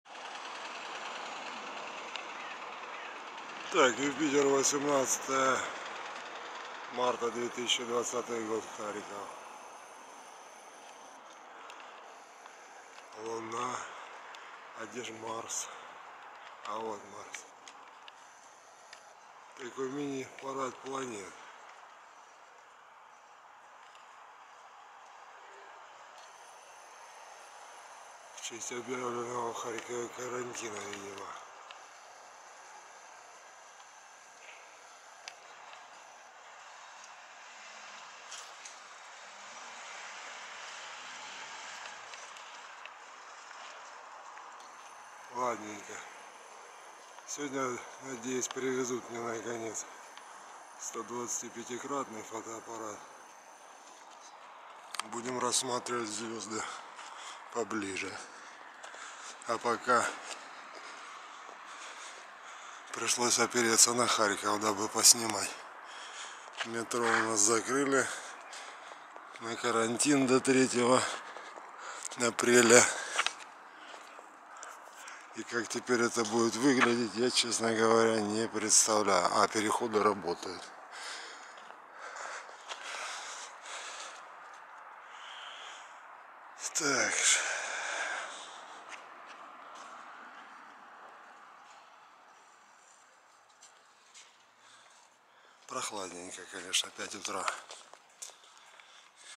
Мини- парад планет: Марс и Юпитер рядом с Луной над Харьковом 18 марта 2020
Урезанный парад планет - Марс и Юпитер очень близко к Луне на небосводе 18 марта 2020 года над Харьковом.